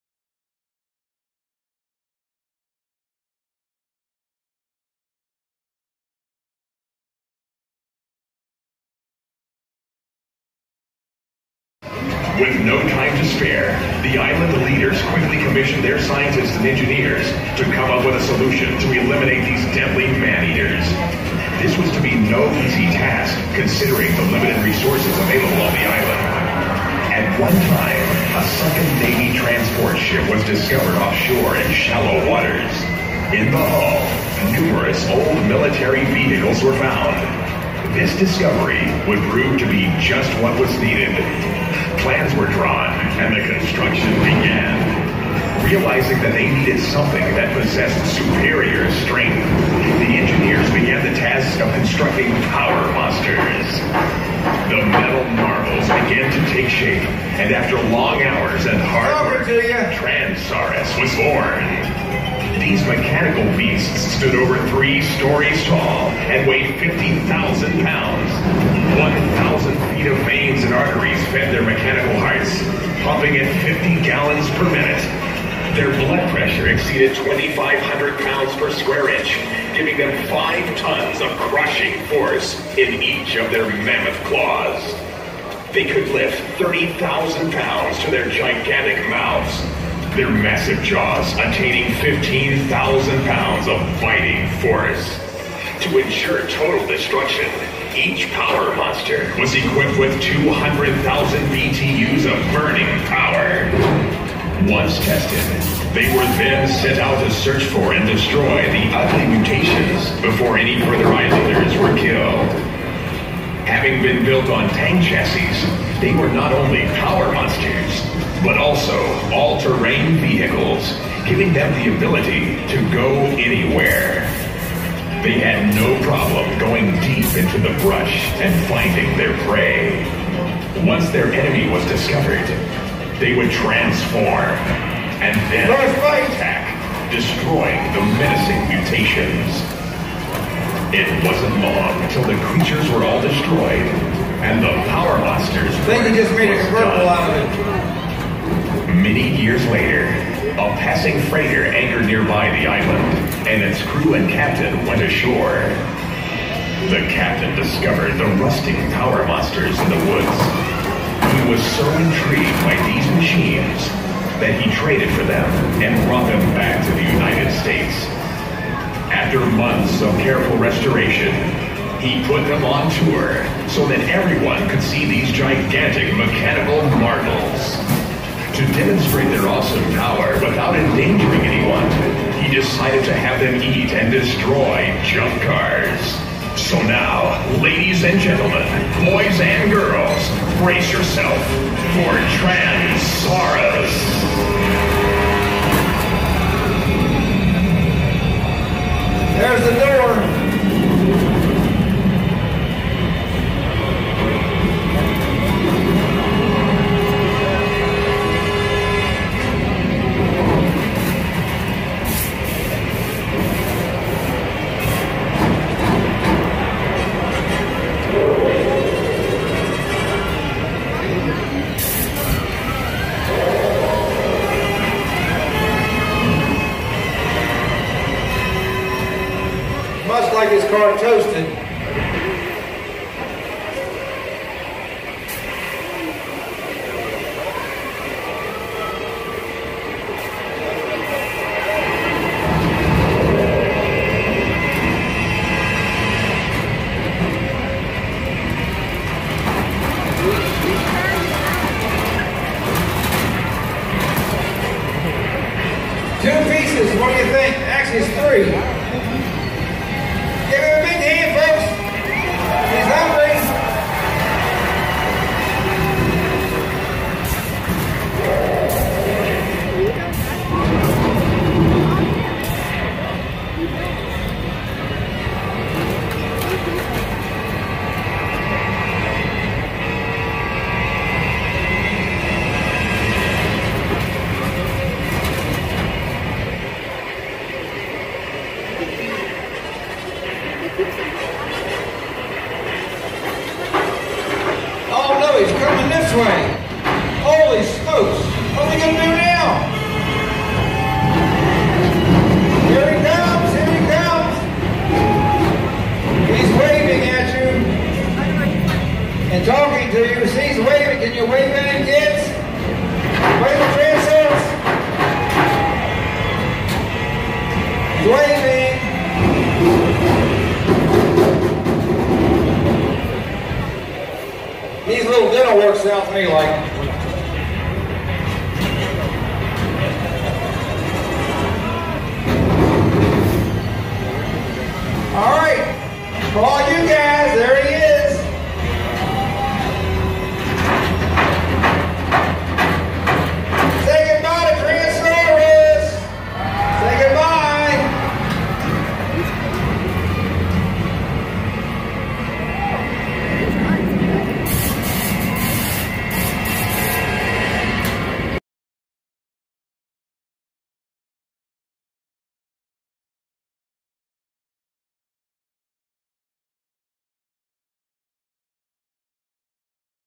2019 CARLISLE ALL TRUCK NATIONALS TRANSSAURUS
This is the 2019 Transsaurus show at the 2019 Carlisle all truck nationals
Gather the kids, sit and enjoy!